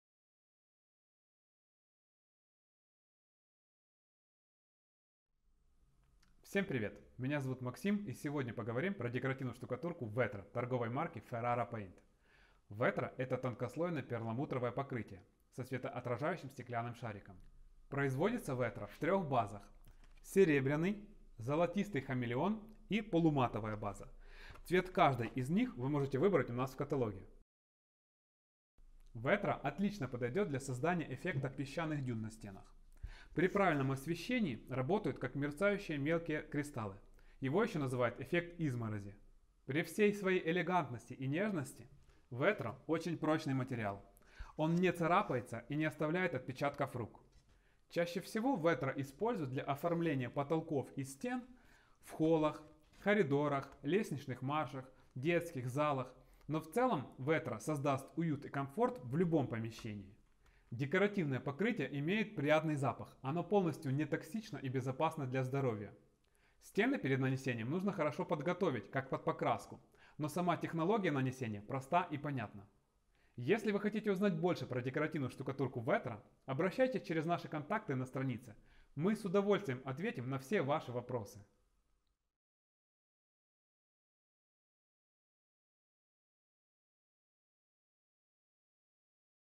Декоративна штукатурка Vetro - ™Ferrara Paint
Декоративне перламутрове покриття на акриловій основі зі скляними сферичними наповнювачами, володіє світловідбиваючими ефектом. Поєднання перламутру та часточок скла, допомагають створити неповторну красу пісчаних дюн та іскристих переливів на ваших стінах.                                                                                                                                  Перевагами цього матеріалу є зручність в роботі, приємний аромат, та помірна ціна. Готове покриття, після повної полімеризації, не осипається та піддається вологому прибиранню.

*Орієнтовна ціна готового покриття від 4,5євро/м2 (залежить від кольору, техніки нанесення, вибору грунт-фарби, фасування матеріалу)                                                                                                                           **1 л. Vetro вистачить приблизно на 7 м² (залежить від техніки нанесення, якості підготовки поверхні)
*** Перед нанесенням Vetro, на поверхню  потрібно наносити кварцеву грунт-фарбу Pittura all Quarzo (яка додатково тонується, при необхідності)